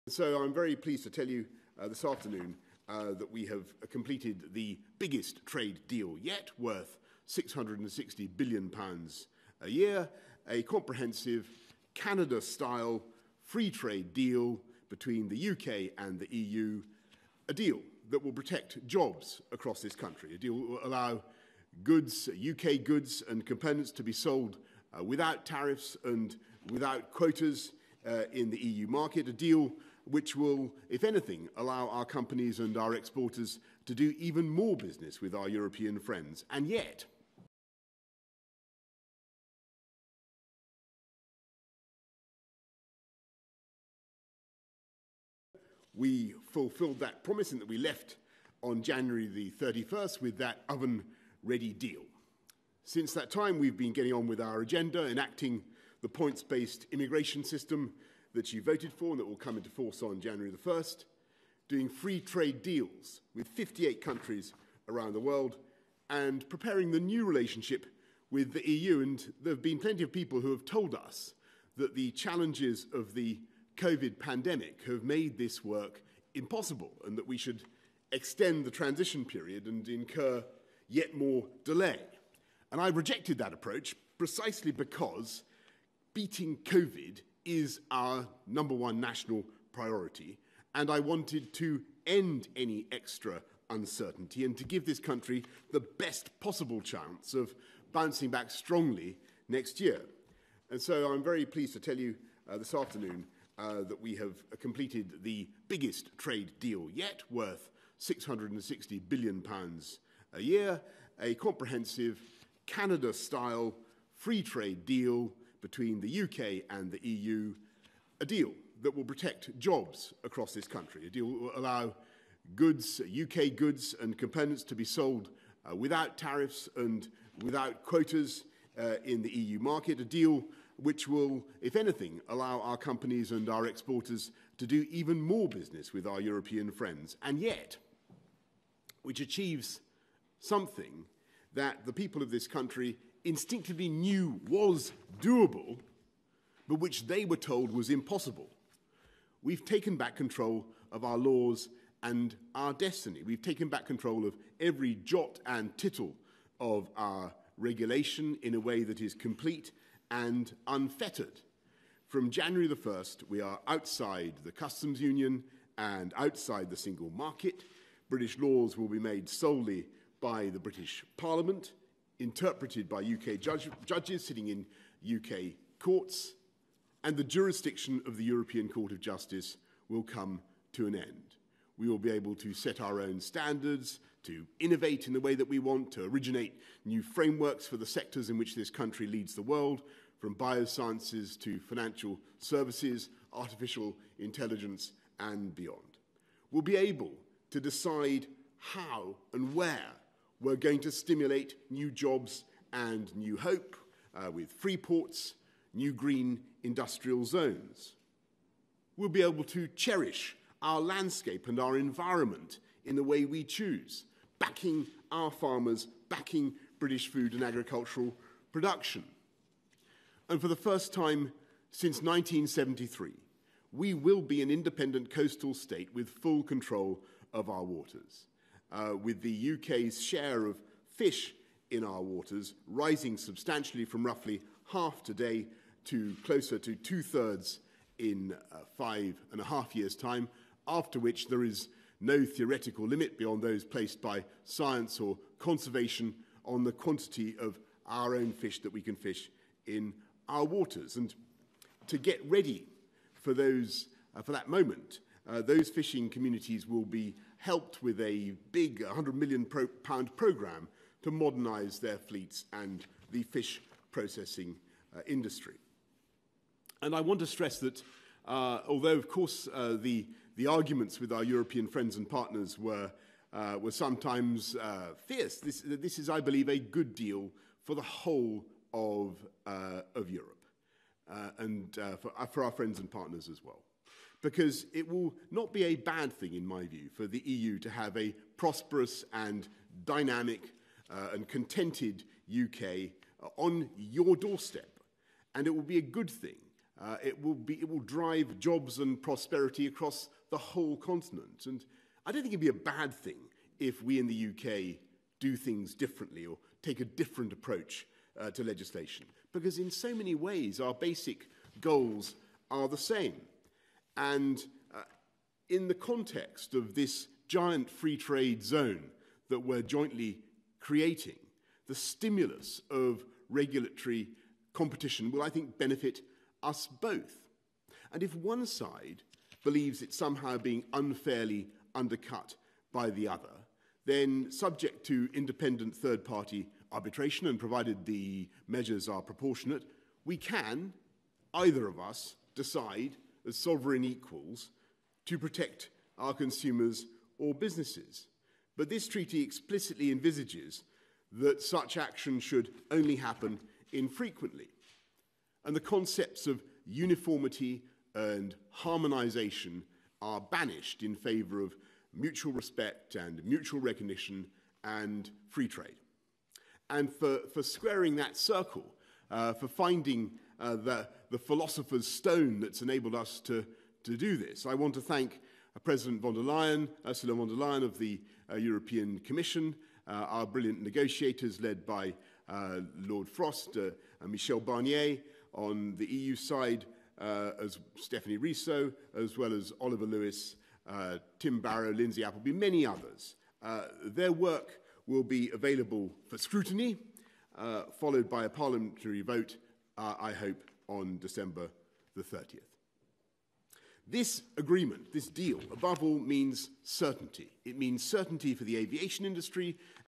LIVE Brexit deal: UK PM Boris Johnson announces HISTORICAL TRADE Agreement with EU. UK- EU DEAL
London(CNN) The United Kingdom and European Union have reached a post-Brexit trade agreement after months of fraught negotiations, the British government said in a statement Thursday. The breakthrough averts a much-feared "no-deal" scenario that would have sparked economic chaos and risked major disruption to the flow of goods and medicines.

"Deal is done," read a statement from Downing Street. "Everything that the British public was promised during the 2016 referendum and in the general election last year is delivered by this deal."

Talks had been deadlocked for months after the two sides were unable to reach agreement in areas such as fishing quotas, how the UK would use state aid to support British businesses post-Brexit, and legal oversight of any deal struck.

Latest News around the world related to Politics, Sports and Health.